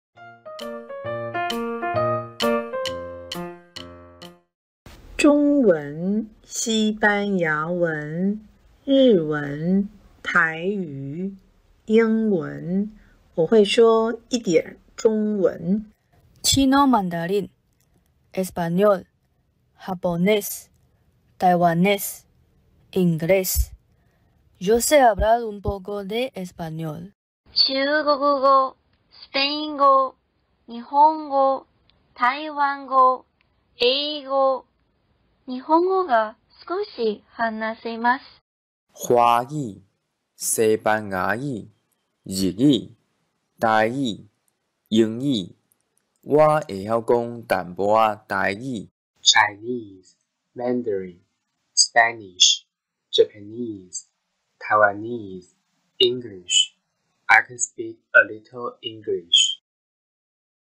我會說一點〇〇篇 🗣️｜GaoWei
面對不熟悉的語言，又想與使用此語言的朋友切磋語言能力，怎麼先開口和對方打個招呼告知你會一點點🤏該語言呢？現在就和我們來練習一下吧！

0:00  （中）我會說一點中文 Wǒ huì shuō yì diǎn Zhōngwén
0:16  （西）Yo sé hablar un poco de español
0:27  （日）日本語が少し話せます Nihonngo ga sukosi hana se ma su
0:38  （台）我會曉講淡薄仔台語 Guá ē-hiáu kóng tām-po̍h
-á Tâi-gí
0:49 （英）I can speak a little English  /ˈaɪ kən ˈspiːk ə ˈlɪtl̩ ˈɪŋ.ɡlɪʃ /